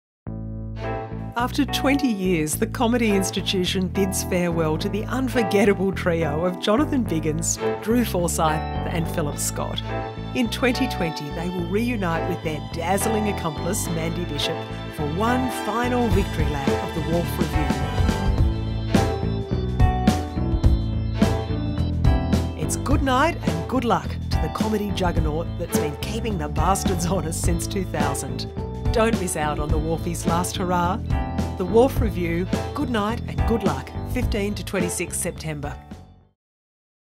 The Wharf Revue - Collected Works 2020
After 20 years the comedy institution The Wharf Revue bids farewell to the unforgettable trio of Jonathan Biggins, Drew Forsythe and Phillip Scott. In 2020 they will reunite with their dazzling accomplice Mandy Bishop for one final victory lap.

It’s good night and good luck to the comedy juggernaut that’s been keeping the bastards honest since 2000.

The bad news is that the planet is on fire and our world leaders are barely qualified to run a chook raffle. And the good? The Wharf Revue has fake news, fake hair and real laughs in abundance. No one is safe and no topic too taboo in this night of sketches, songs and side-splitting satire. You’ll laugh! You’ll cry! You’ll want to move to New Zealand!